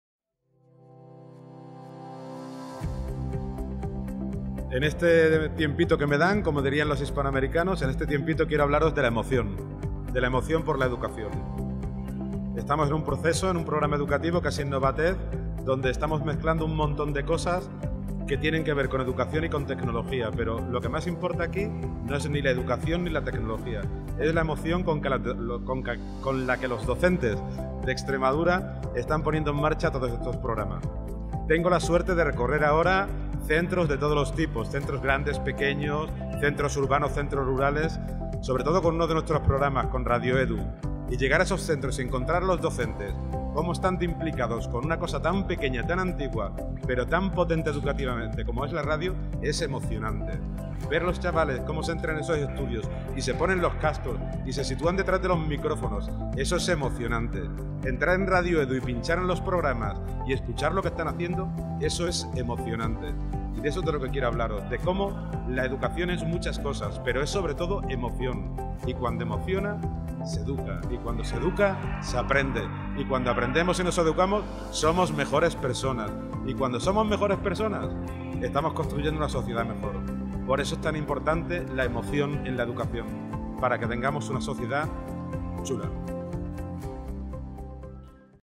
"La emoción educativa" (Francisco López. Servicio de Tecnologías de la Educación)
Radio educativa como ejemplo de cómo cuando se emociona, se educa.